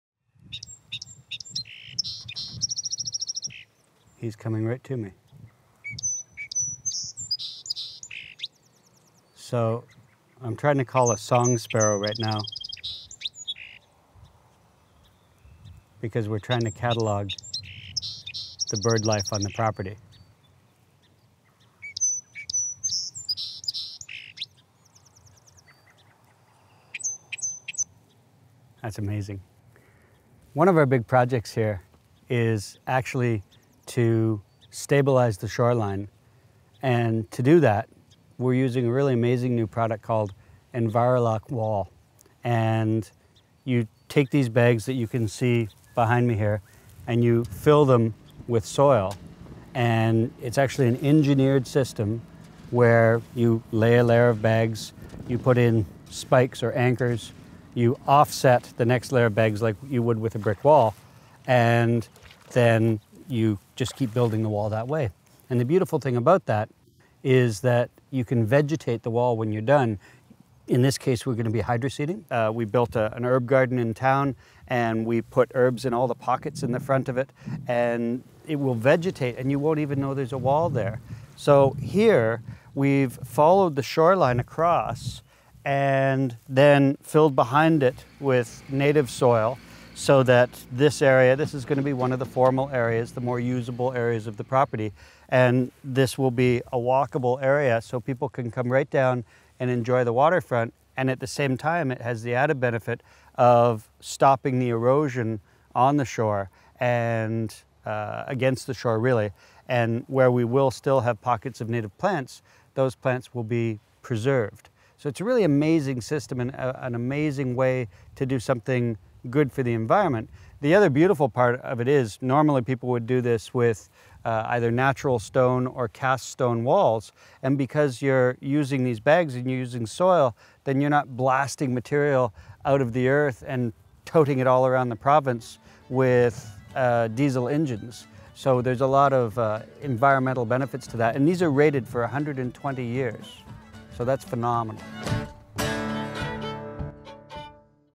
Sean at the Mill Dam Pond - Erosion Control
Fern Ridge Landscaping is on site in Guysburough, Nova Scotia working on stabilizing an eroding shoreline using a product called Envirolok Wall. This is an environmentally friendly method of erosion control.